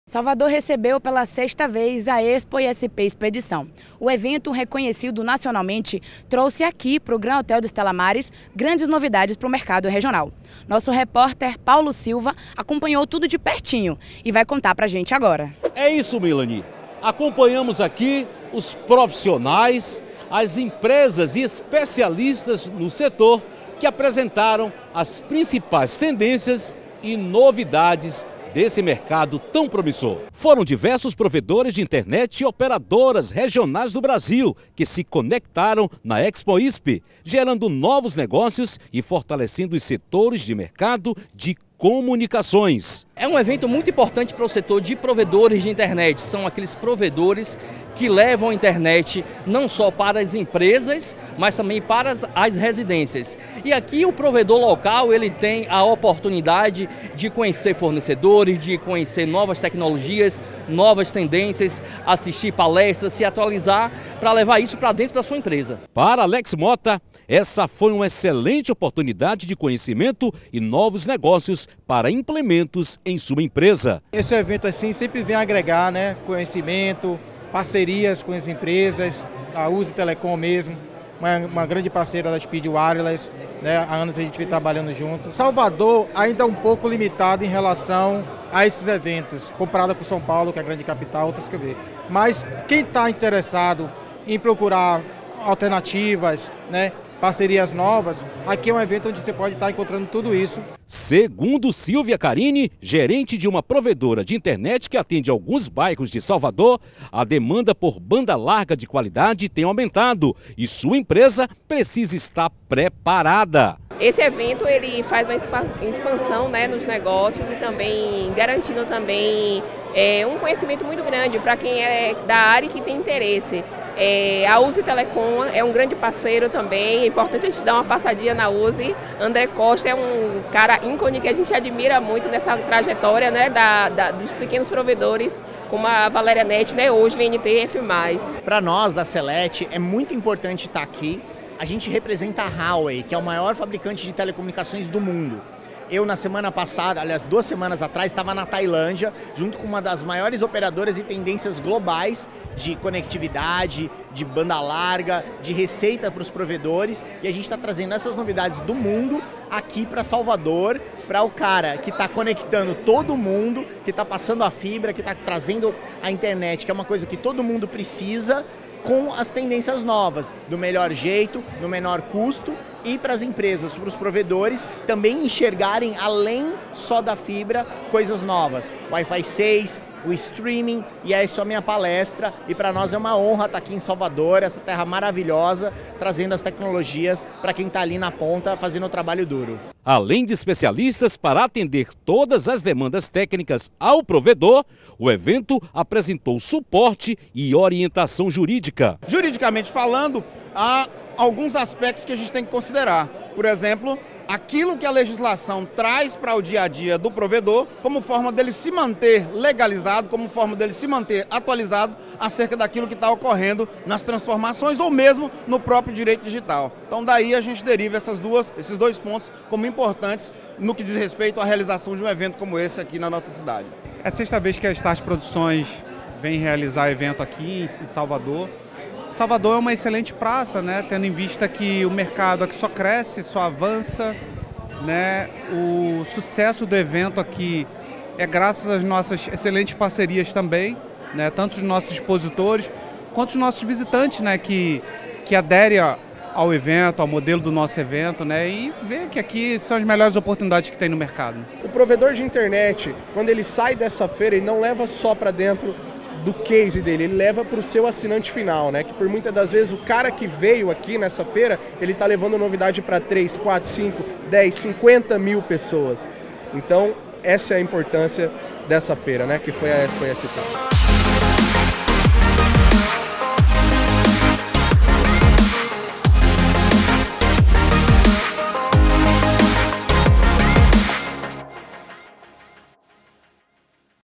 A Burburinho News esteve na EXPOISP Expedição
A Burburinho News esteve na EXPOISP Expedição, evento que leva os principais Provedores de Internet e Operadoras Regionais do Brasil para gerar novos negócios e fortalecer o networking